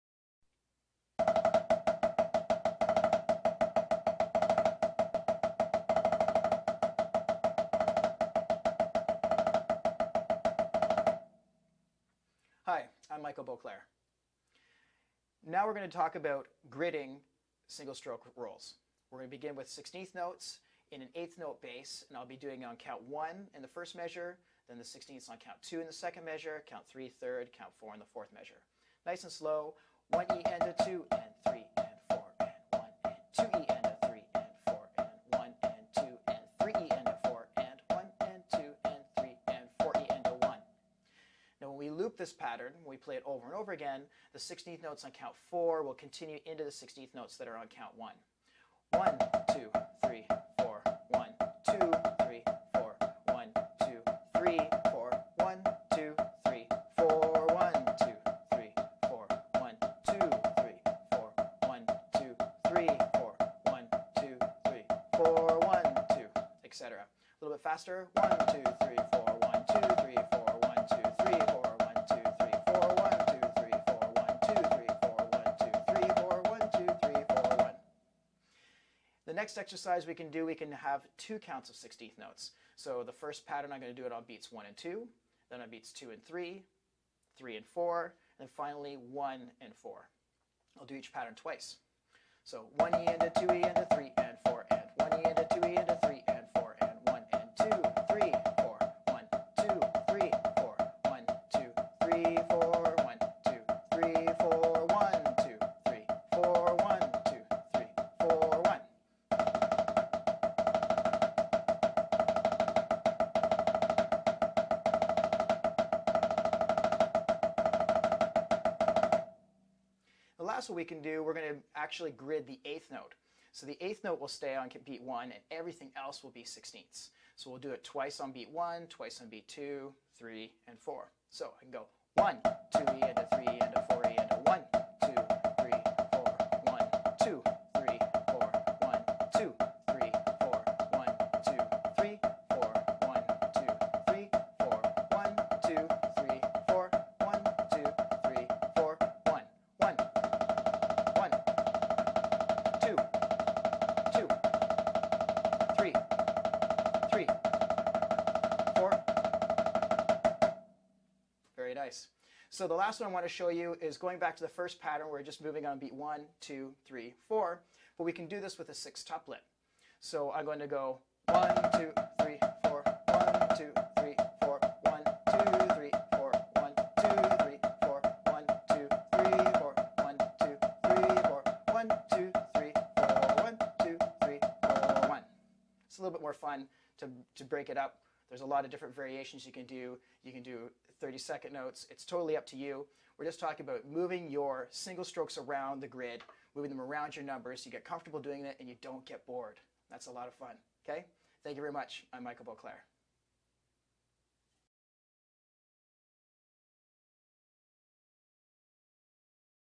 Single Stroke Drum Roll Grids with Michael Beauclerc
Drumline educator Michael Beauclerc demonstrates single stroke roll grids. Drum exercises from his book "The Mad Practice Pad"